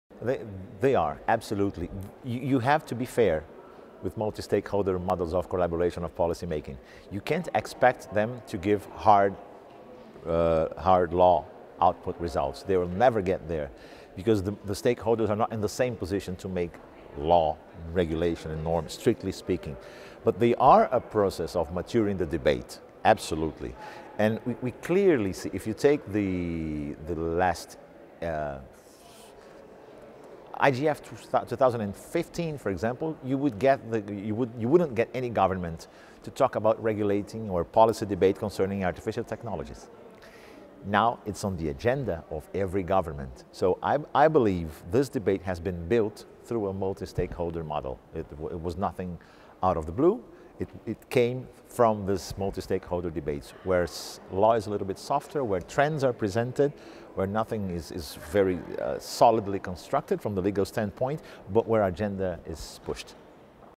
Global IGF 2017: Claudio Lucena on Multistakeholder Model
Claudio Lucena, professor at UEPB Brazil and FCT Portugal, shares their answer to the question, “Do multistakeholder efforts like IGF have any impact on the processes they are created to positively influence?” Recorded at the Global Internet Governance Forum 2017 in Geneva, Switzerland, by the Imagining the Internet Center, an initiative of Elon University, North Carolina, USA.